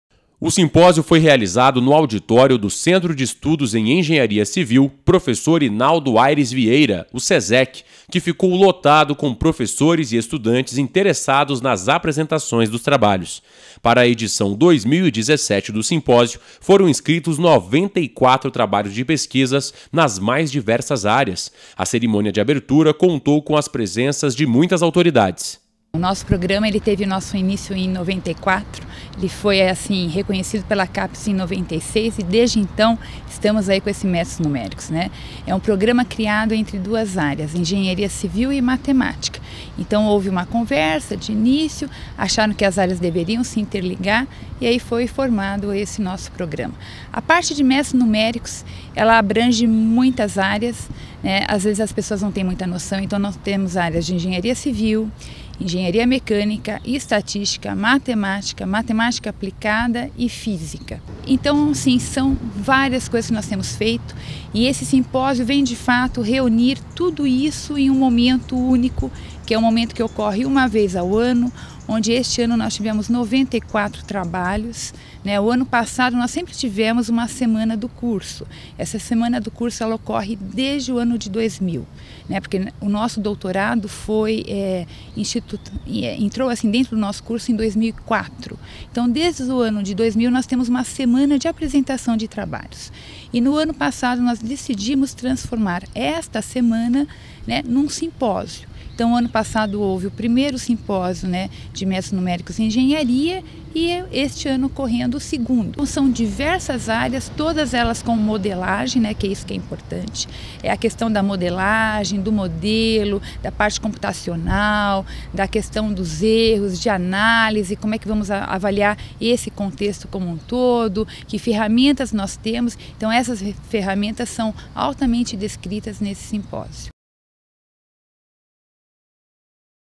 Informa | Simpósio de Métodos Numéricos em Engenharia
Nesta semana, a UFPR recebe o II Simpósio de Métodos Numéricos em Engenharia (SMNE 2017). De 25 a 27 de Outubro de 2017. O evento debate os desenvolvimentos científicos recentes na área, além de promover a colaboração entre investigadores, engenheiros, especialistas e estudantes.